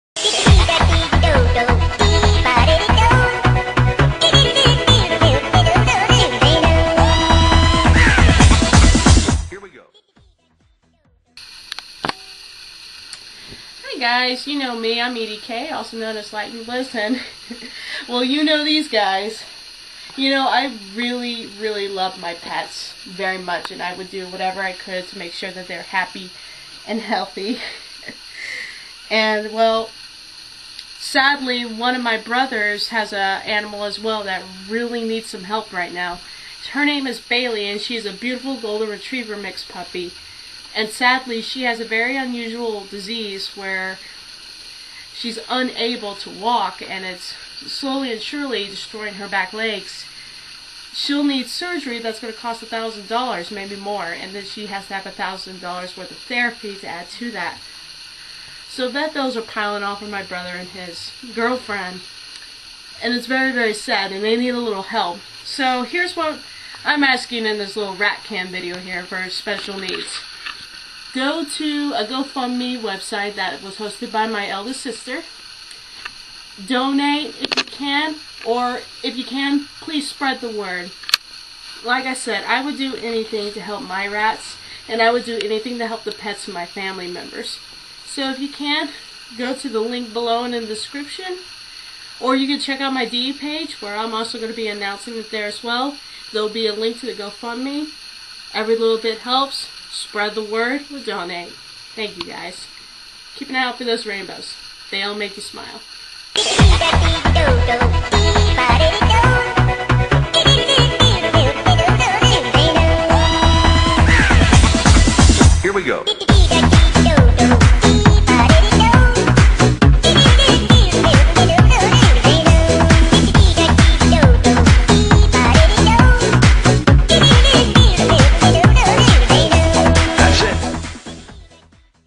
Rat Cam 98 - Help Save Bailey
Please help to save Bailey, a Chihuahua mix who was adopted by my brother and his girlfriend this year. Bailey has "Perthes Disease" and is currently on meds to help cope with her agonizing pain.  She will need to undergo surgery, followed with physical therapy. My brother's family has gone through so much this past year and could really use your help.  Please consider donating what you feel comfortable with or help spread the word. Every little bit helps!

Help Save Bailey Go Fund Me: - Fund has ended -